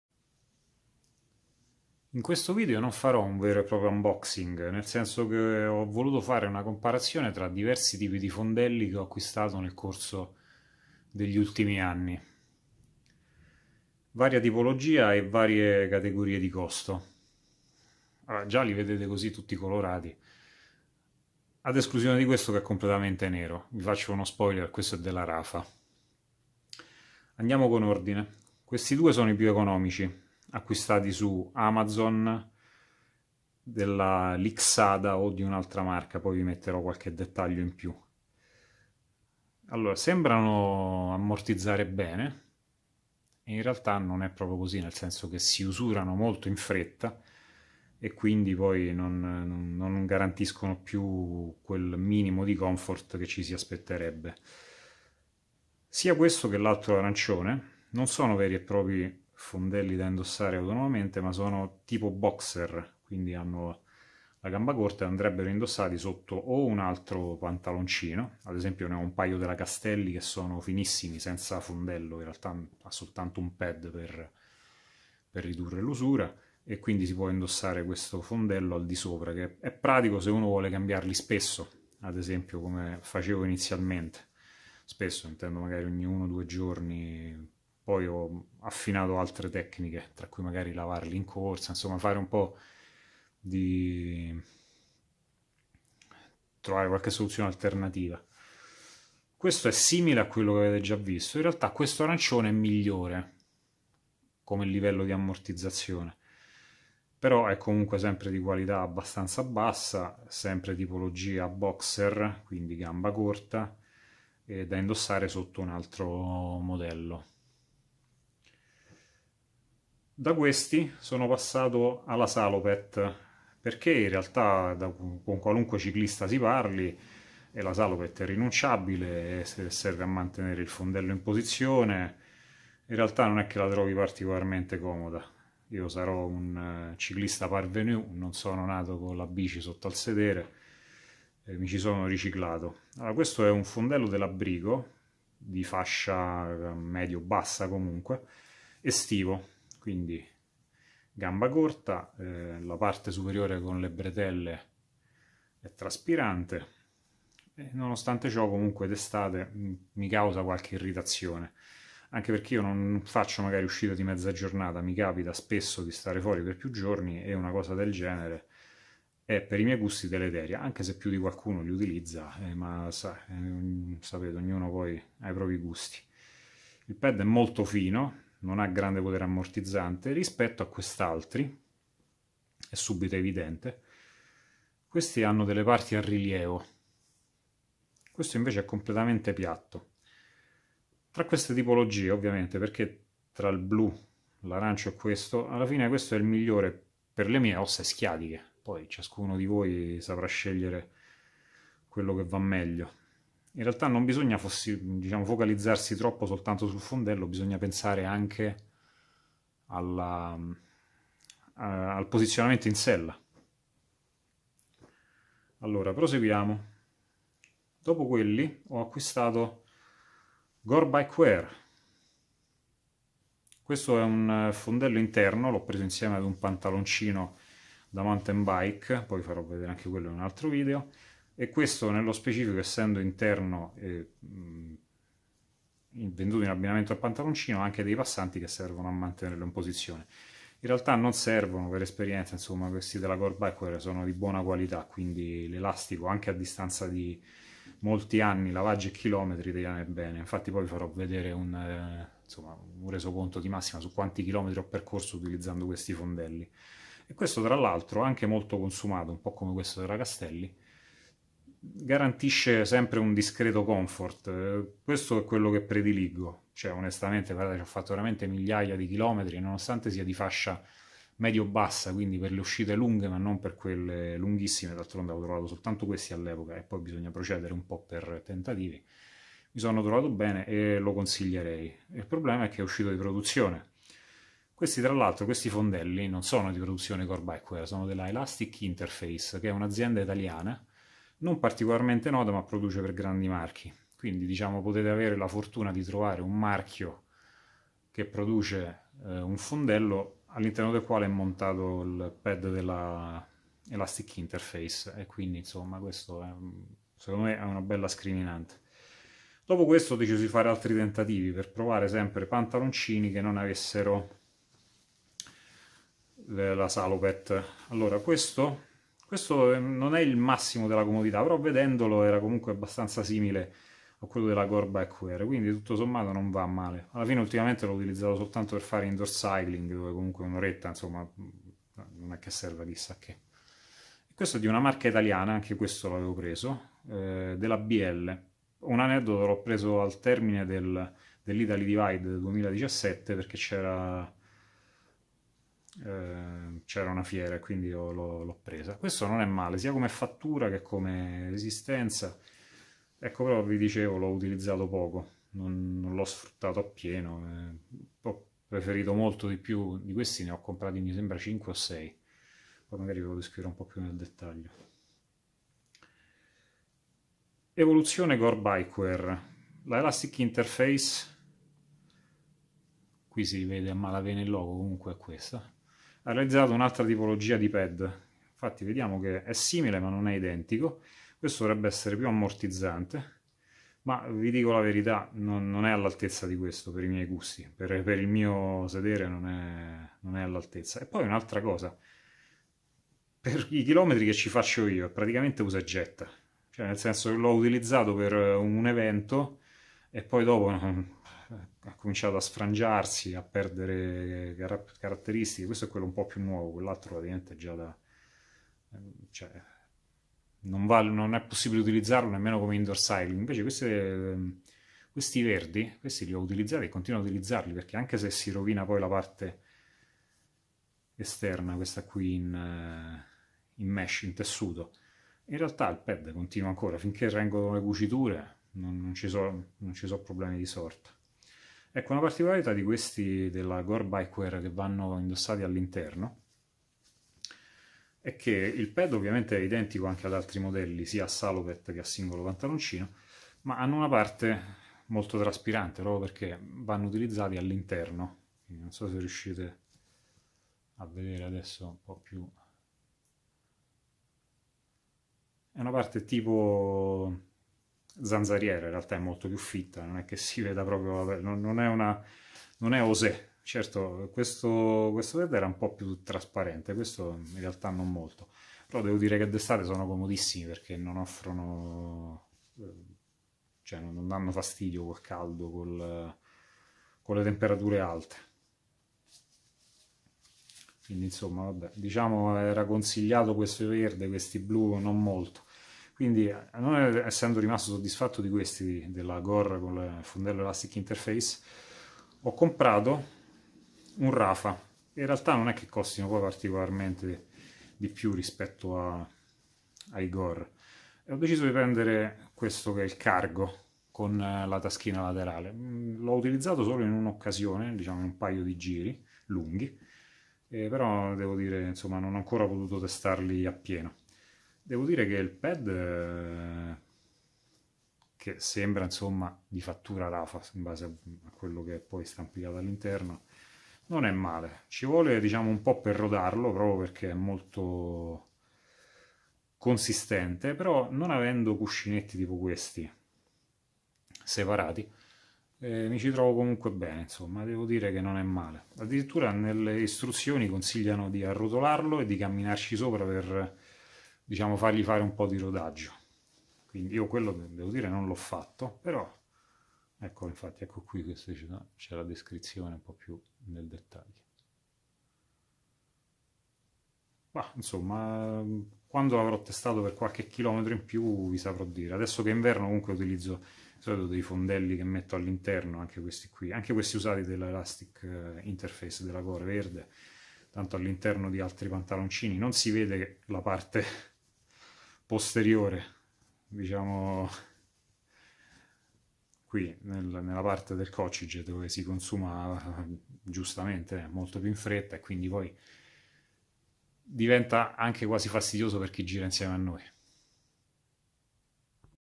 Fondelli e sottopantaloncini per ciclismo - chamois, bike pads and cycling pads
Segui la pagina Facebook e gli account Instagram e YouTube per non perdere i nuovi articoli

A short review on Gore Bike Wear shorts with Elastic Interface pads, Briko salopette with pad, BL cycling shorts, Rapha Core Cargo shorts, Aliexpress and Amazon cycling underwear